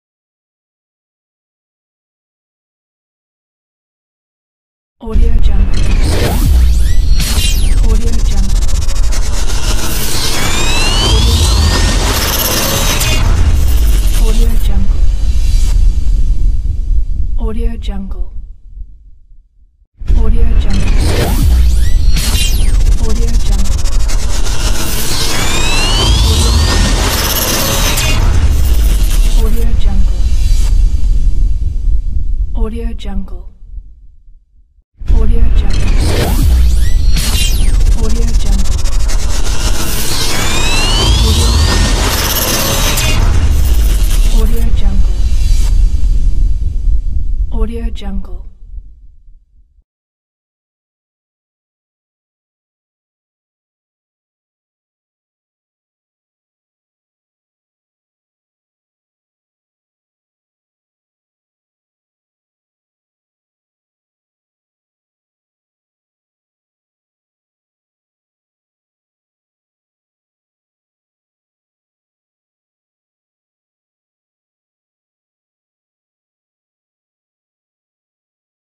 Energy Logo Reveal Free After Effects Templates
MAIN FEATURES:

Requires free plugin Saber.
Custom Color Control.
1 Logo Placeholders, 1 Text Placeholders.
Full HD Resolution (1920×1080)+(1280×720) 30fps, Composition included.
The Sound FX file is included with the project files.